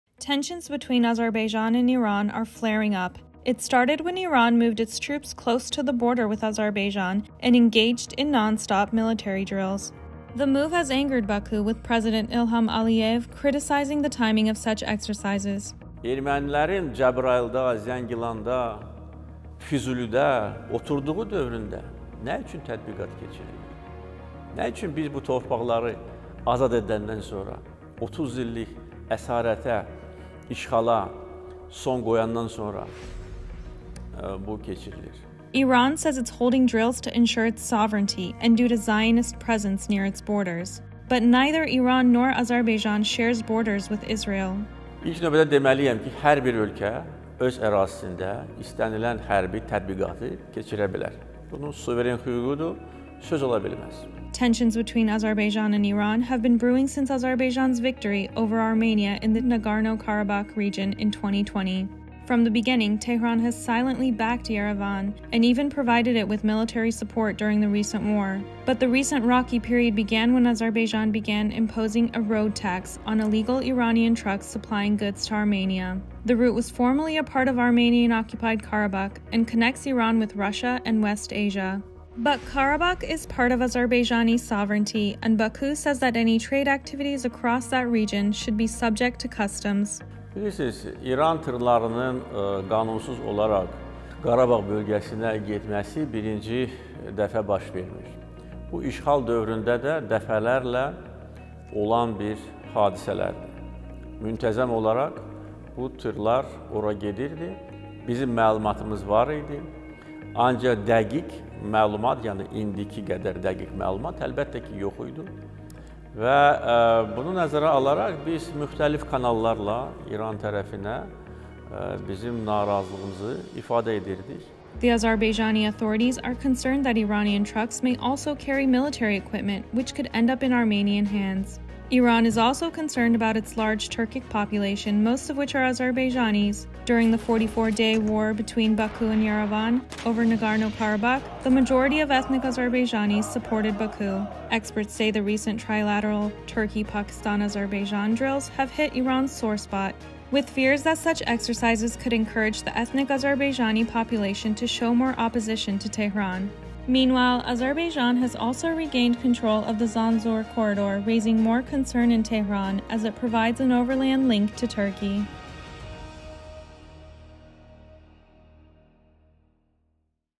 What’s behind the escalation of tensions between Azerbaijan and Iran?
Tensions between Azerbaijan and Iran have escalated after Iran moved its troops close to the border with Azerbaijan and has engaged in non-stop military drills.